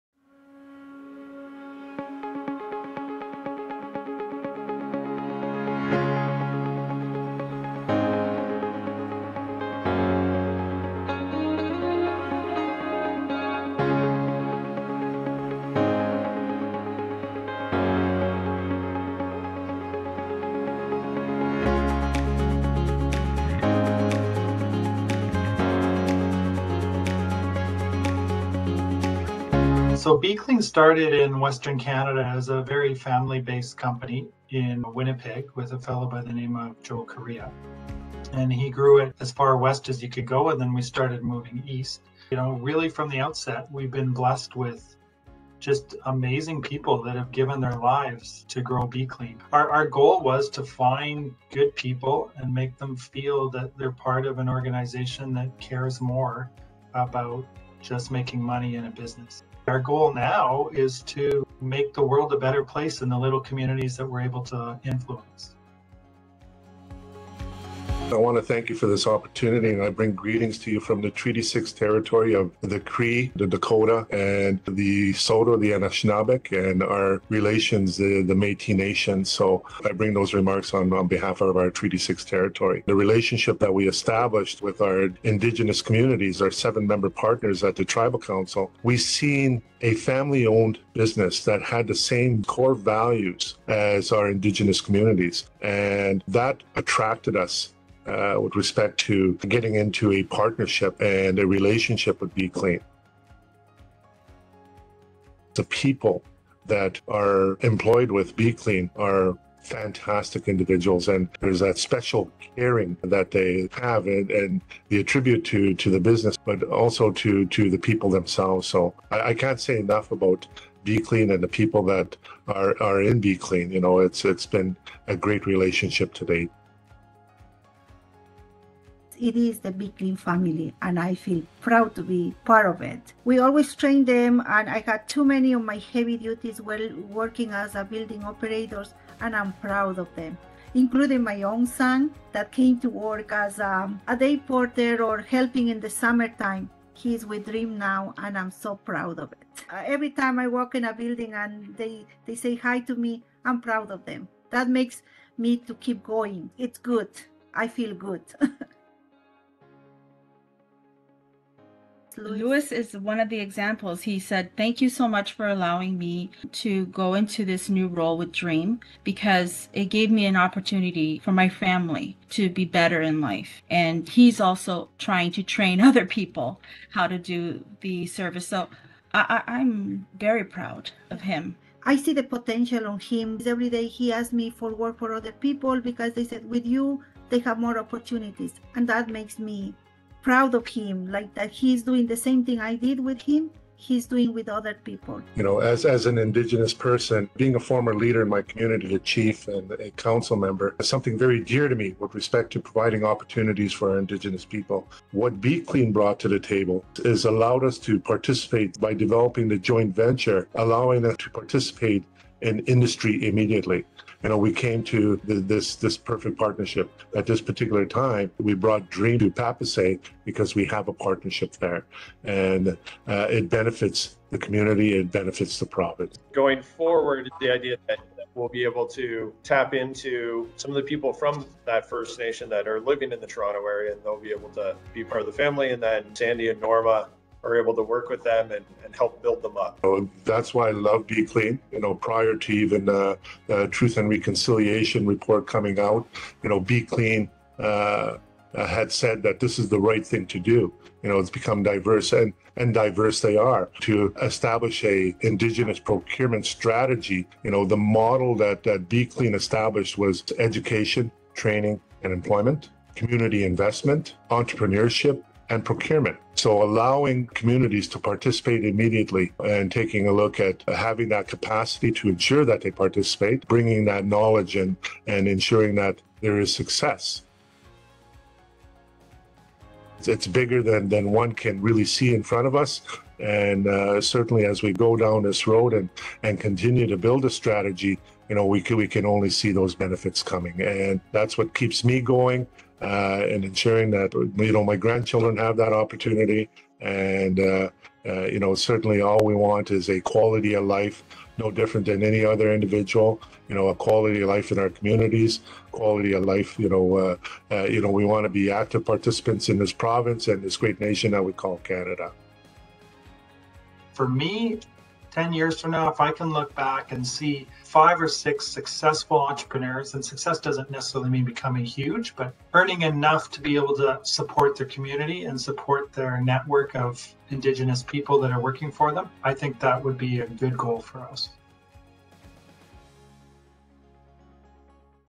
Bee Clean - Social Procurement Strategy
Bee-Clean has been an important cleaning service provider for Dream since 2001. In response to our Social Procurement Strategy, they extended their partnership with Papasay, the Economic Development Corporation owned 100% by Sand Point First Nation, to provide better opportunities to Indigenous communities.

In our recent interview, Brad Keast, Head of Development and Development Construction of Dream Office REIT, sat down with Bee-Clean's team to learn more about how their joint 51/49 Indigenous partnership called Papasay Bee-Clean is impacting and benefiting their employees.